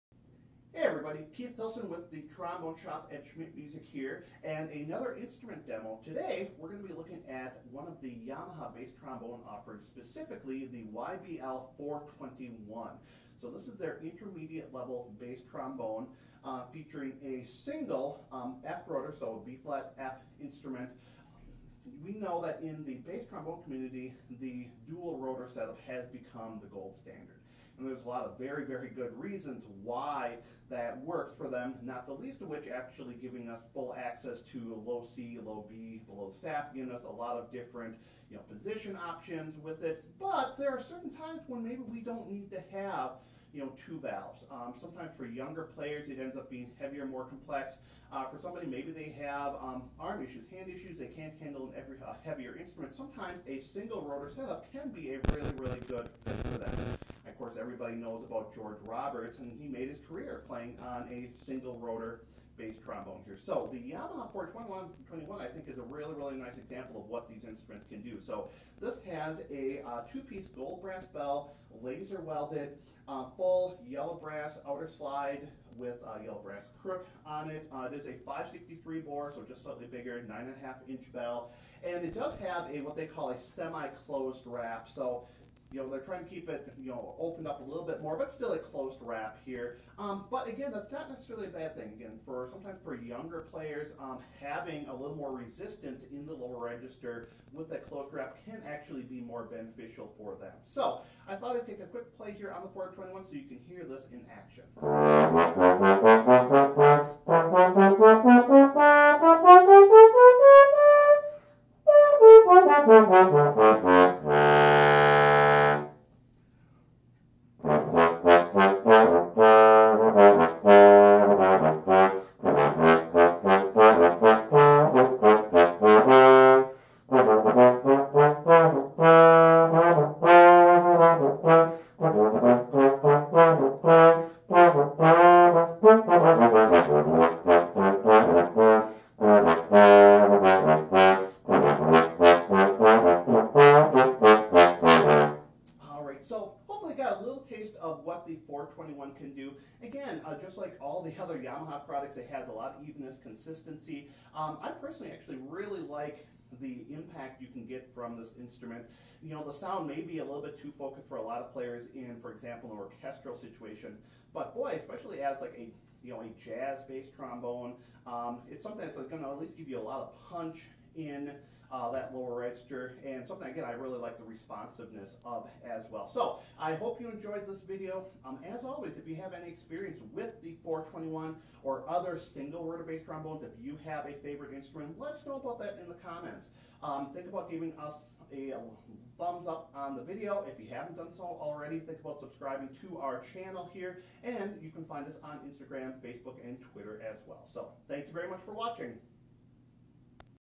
Yamaha YBL-421G Bass Trombone Demo
Another play-through on a visiting trombone from Yamaha; this week, the YBL-421G.  A nice intermediate option for the player looking for a single-rotor, good-sounding, easy-responding bass trombone.  It felt only right to pull out a little George Roberts on this instrument!